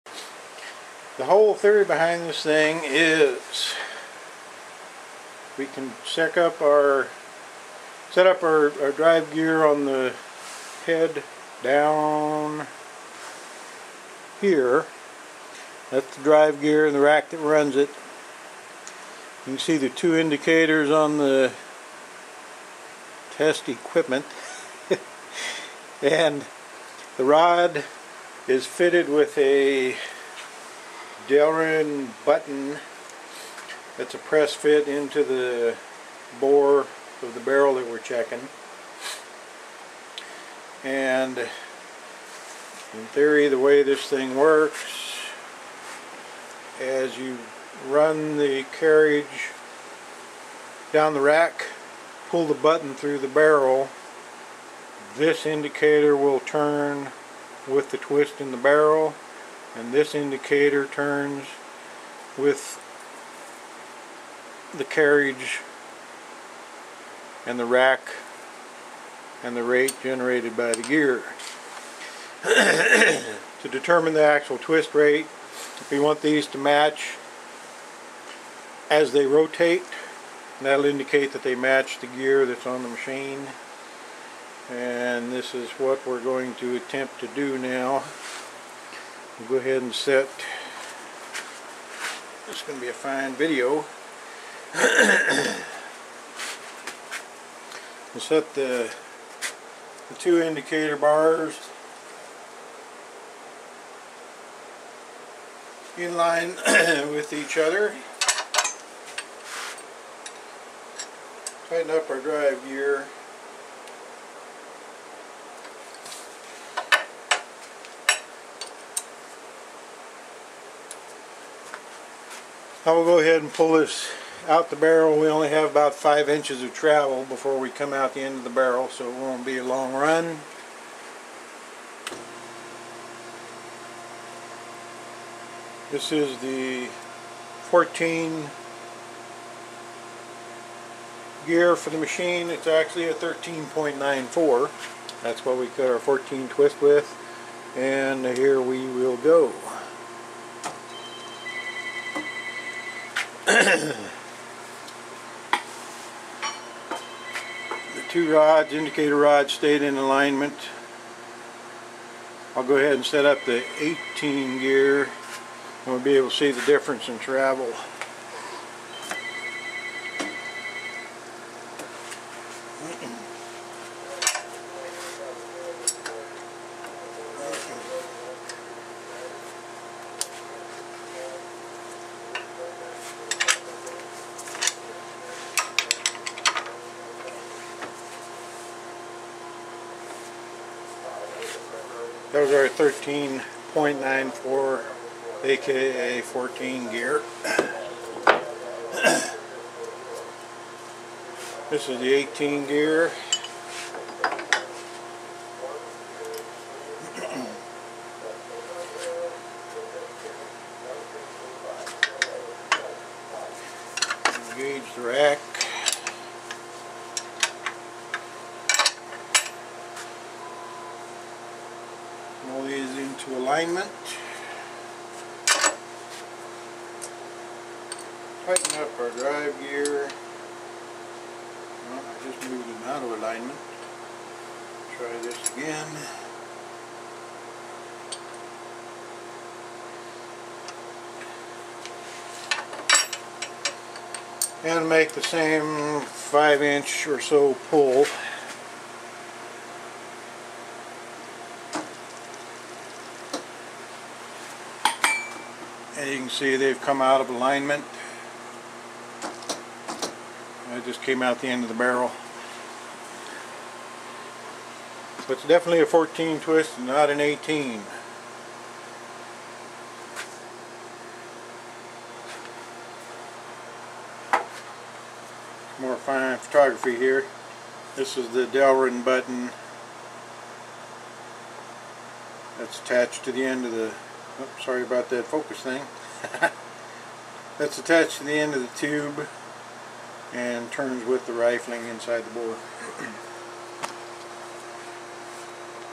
checking twist rate
Determining twist rate in a rifled bore using the rifling machine at Clearwater Reboring and "Uncle Dad's Whiz-Bang Chicken Pluckin'-Twist Rate Measurin' Gizzie Model-III '